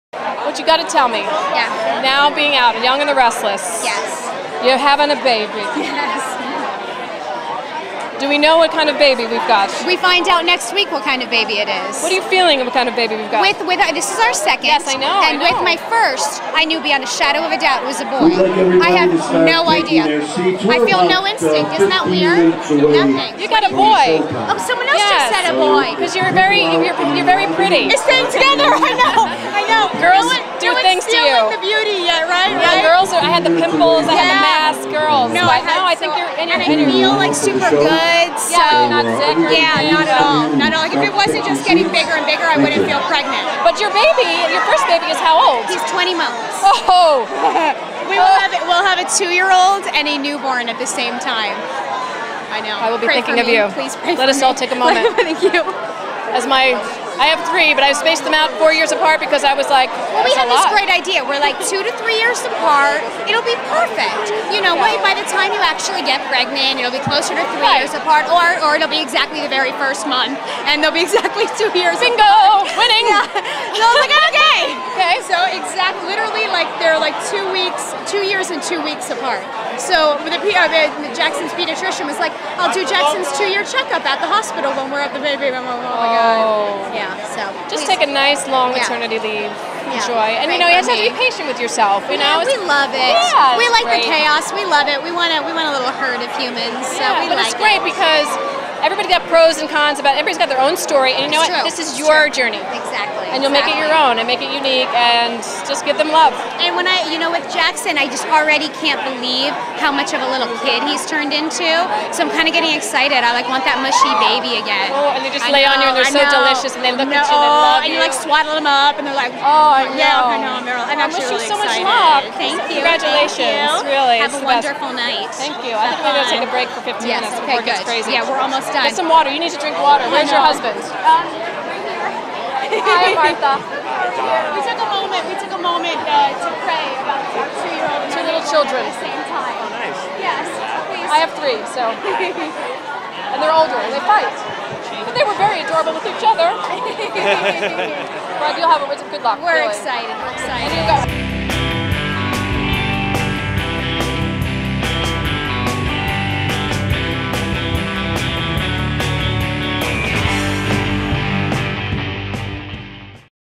2011 Daytime Emmys | Marcy Rylan
A Red Carpet interview with The Young and the Restless' Marcy Rylan. This interview was conducted at the Las Vegas Hilton in Las Vegas, Nevada on June 19, 2011.